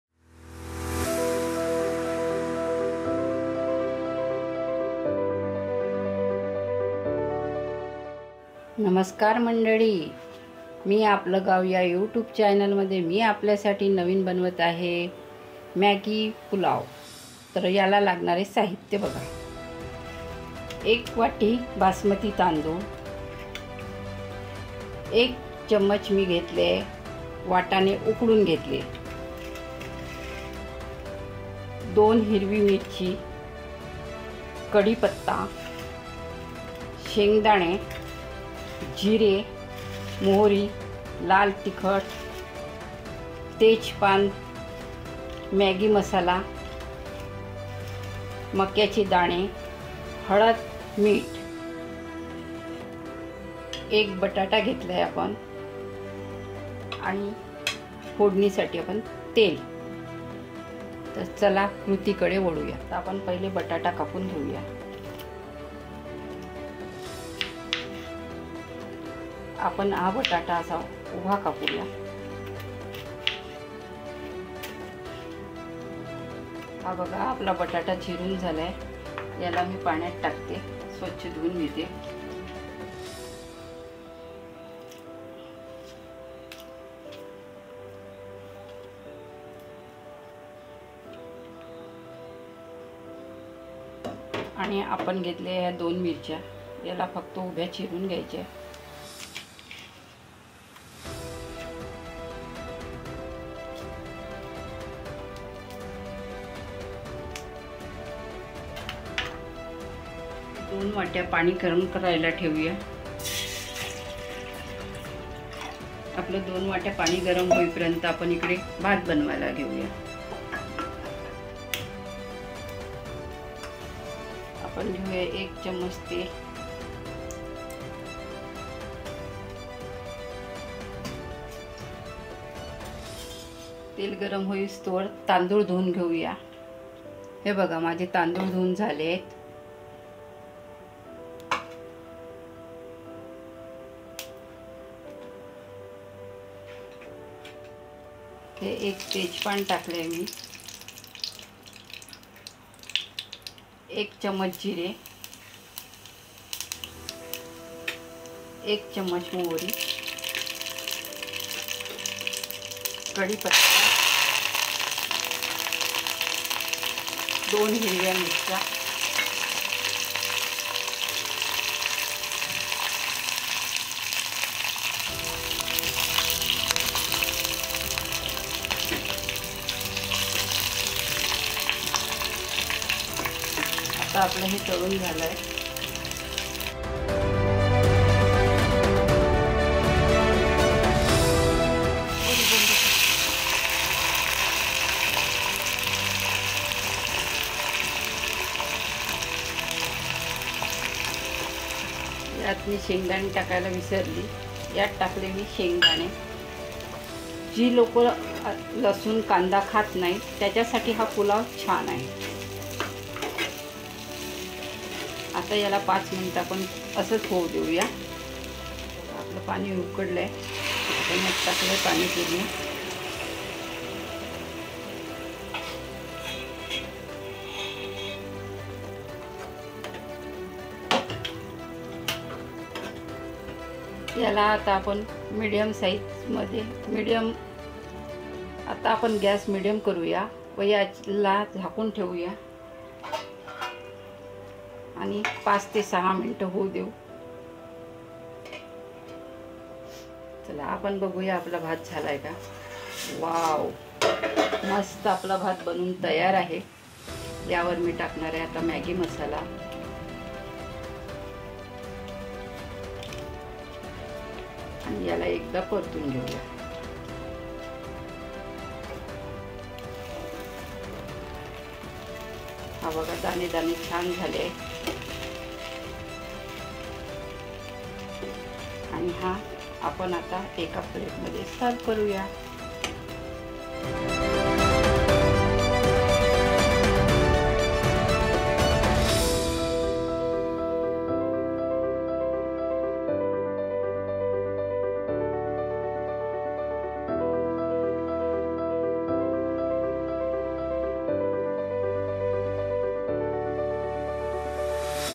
Maggi pulao | tasty pulao recipe | unique pulao idea | how to make Maggie pulao | मॅगी पुलाव
आज तुमच्यासोबत मॅगी पुलाव सांगणार आहे, तो खायला खूप चविष्ट दिसतो आणि बनवायलाही सोपा आहे, त्यामुळे तुम्ही नक्कीच बनवाल, मला आशा आहे की तुम्हाला हा व्हिडिओ आवडेल.